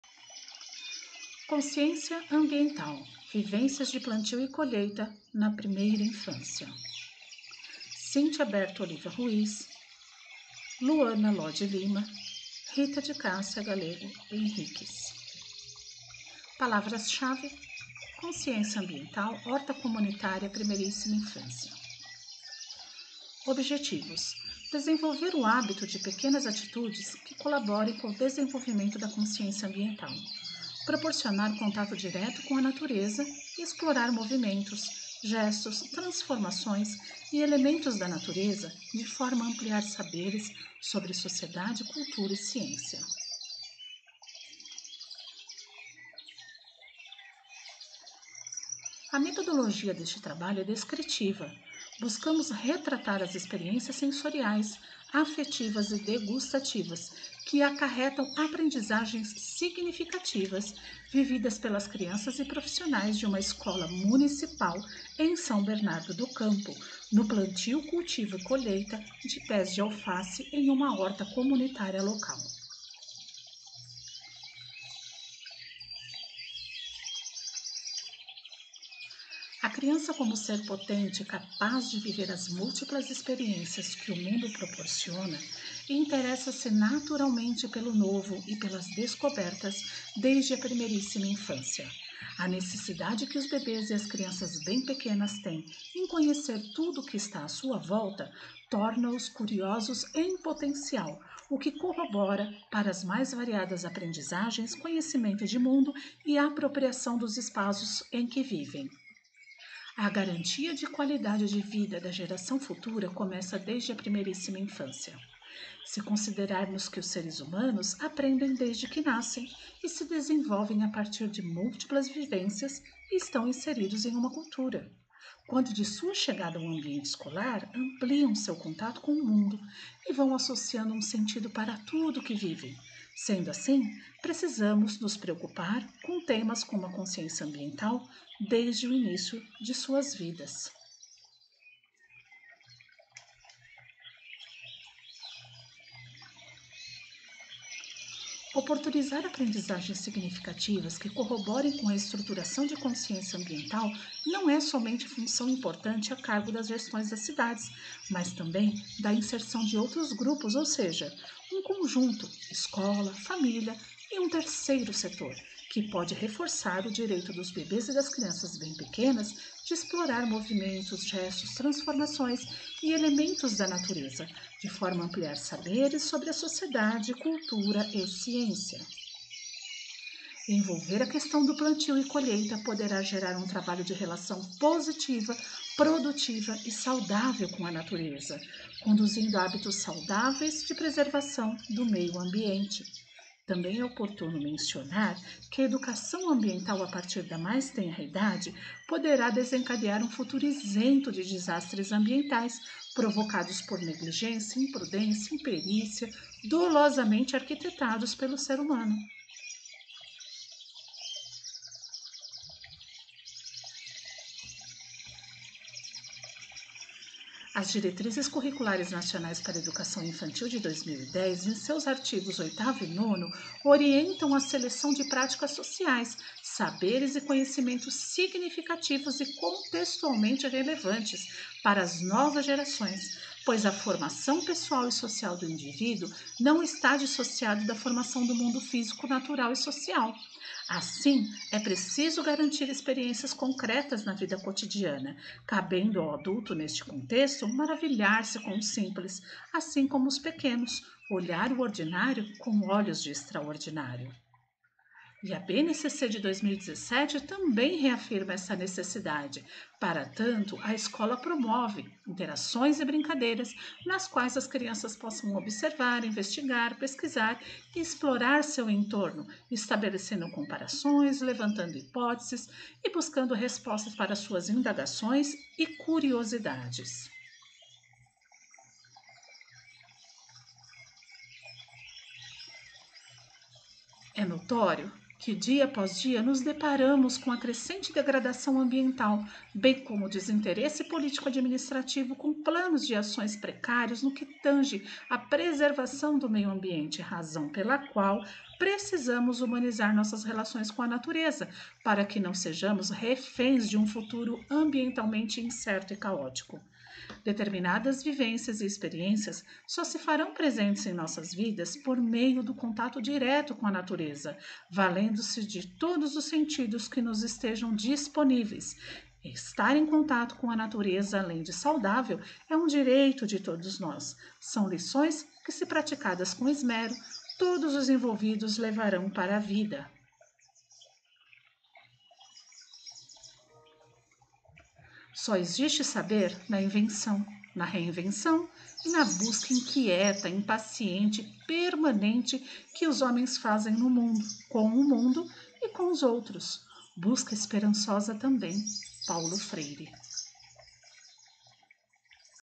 CONSCIÊNCIA AMBIENTAL: VIVÊNCIAS DE PLANTIO E COLHEITA NA PRIMEIRÍSSIMA INFÂNCIA.
Título do trabalho: CONSCIÊNCIA AMBIENTAL: VIVÊNCIAS DE PLANTIO E COLHEITA NA PRIMEIRÍSSIMA INFÂNCIA.

Autores: Rita de Cássia Gallego Henriques; Cintia Berto Oliva Ruiz; Luana Lodi Lima.